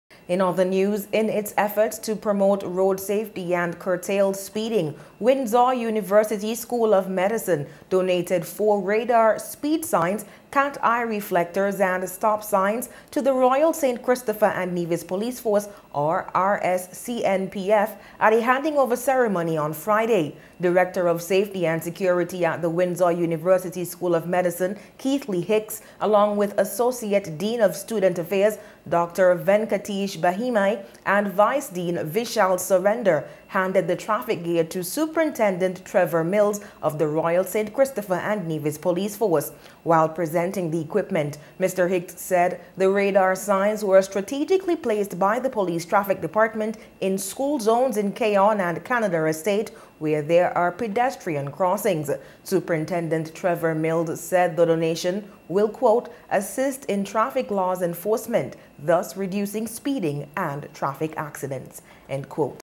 Windsor donates to police
Windsor University School of Medicine Donates Road Safety Equipment to the Police